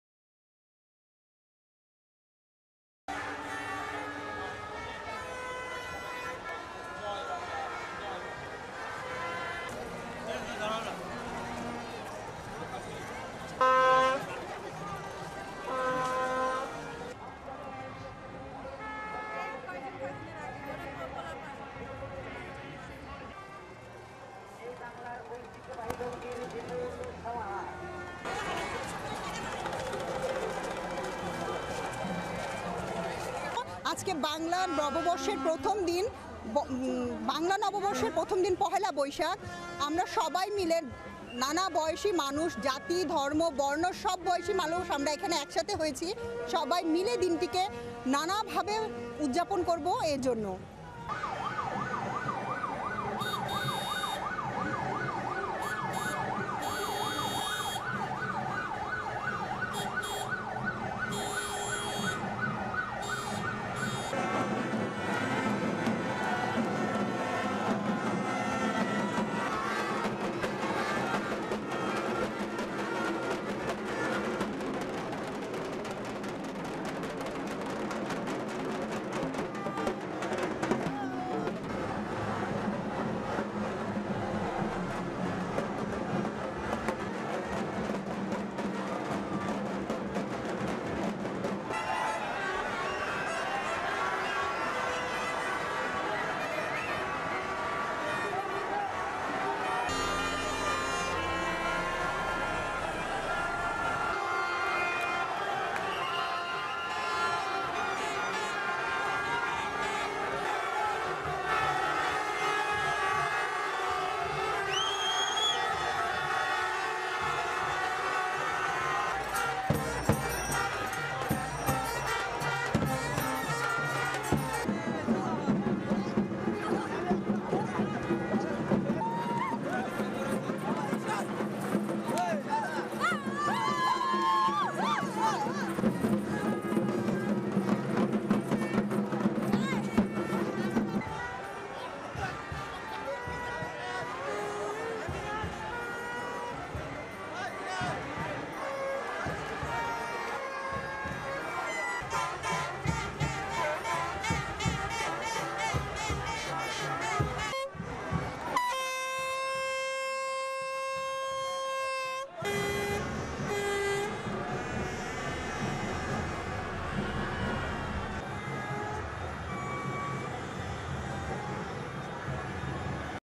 15 April, 2015 - Bangladeshis welcome 'New Year'
15 April, 2015 (ANI) - Thousands of Bangladeshis, old and young alike, clad in colourful clothes, throng the streets of Dhaka city to celebrate 'Poila Baisakh' or Bengali New Year.